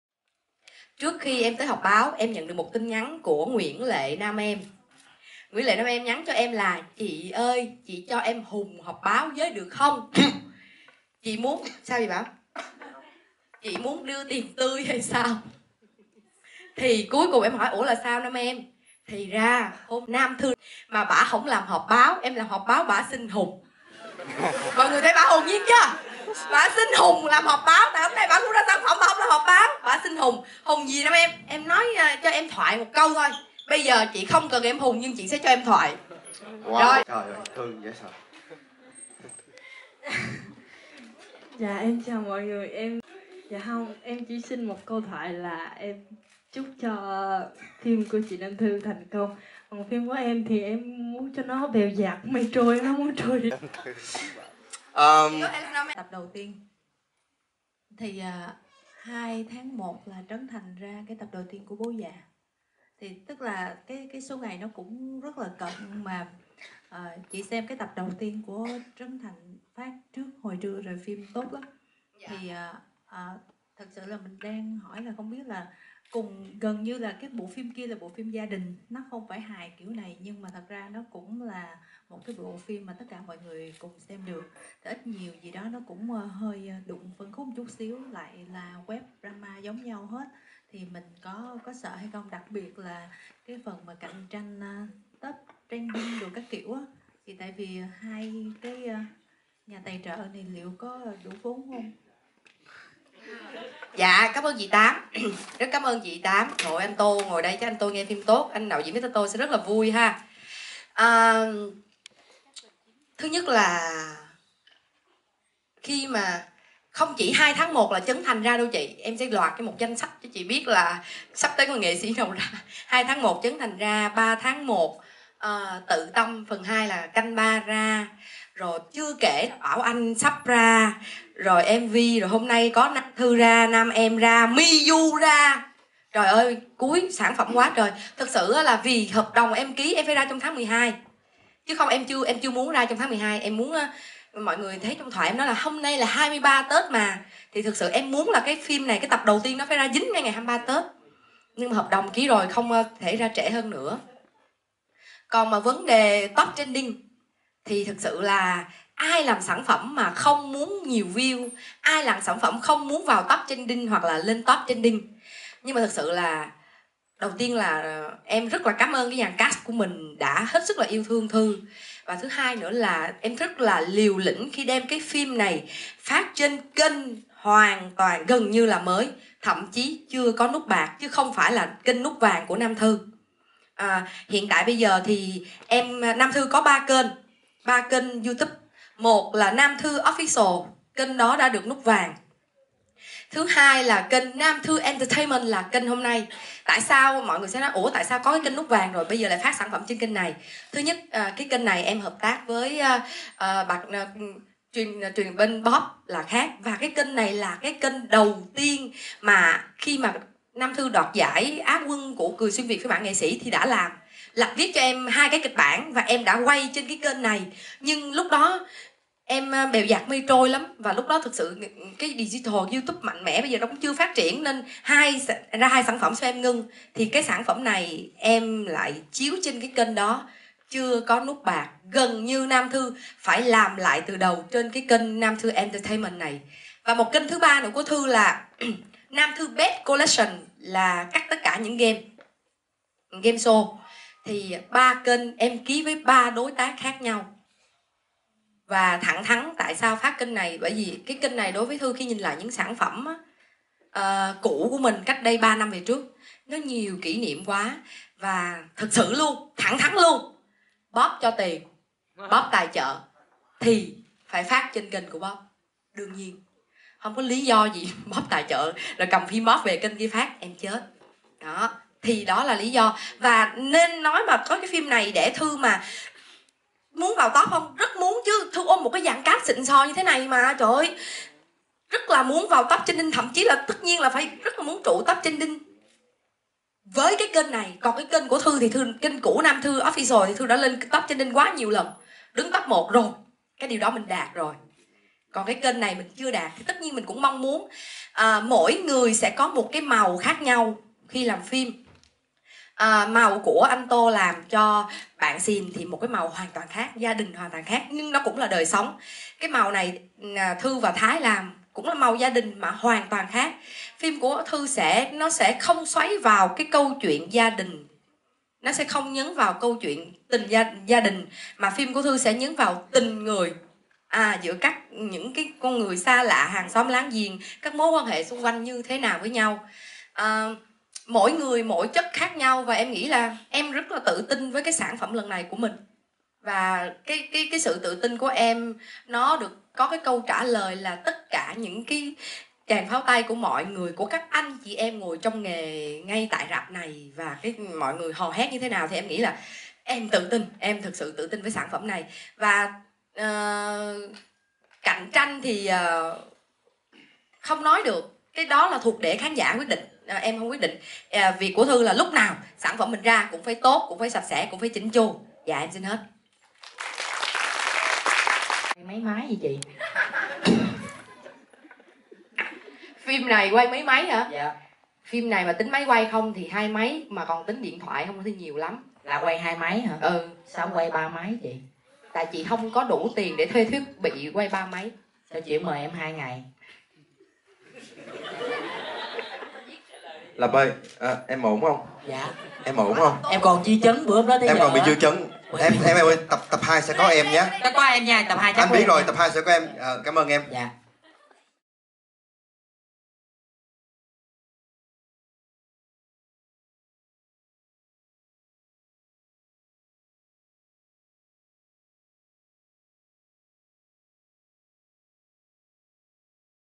Nam Thư tự tin dù ra phim "đụng" Trấn Thành, tiết lộ đề nghị bá đạo của Nam Em
Nam Thư tự tin dù ra phim NHÀ TRỌ CÓ QUÁ TRỜI PHÒNG "đụng độ" phim BỐ GIÀ của Trấn Thành, tiết lộ đề nghị bá đạo của Nam Em

Kênh SaiGon Stories là kênh chuyên về giải trí showbiz và đời sống